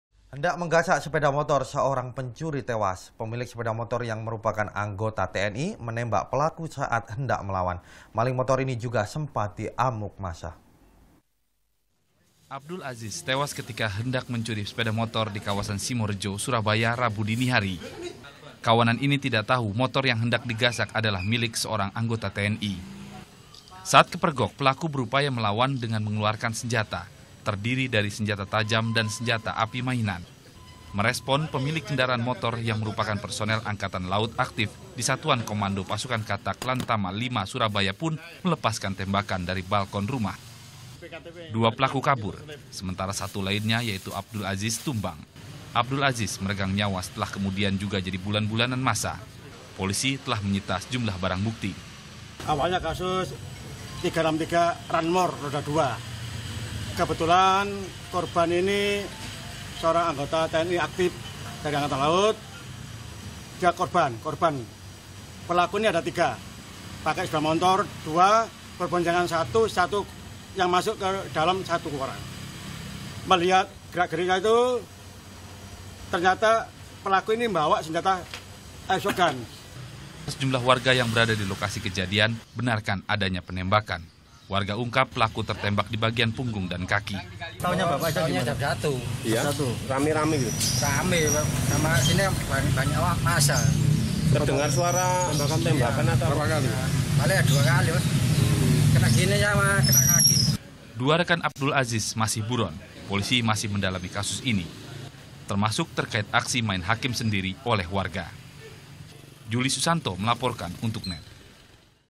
NET. JATIM - KORBAN RANMOR BERHASIL TEMBAK PELAKU
** NET. JATIM **
Hendak Menggasak Sepeda Motor, Seorang Pencuri Tewas. Pemilik Sepeda Motor Yang Merupakan Anggota Tni, Menembak Pelaku Saat Hendak Melawan. Maling Motor Ini Juga Sempat Diamuk Massa.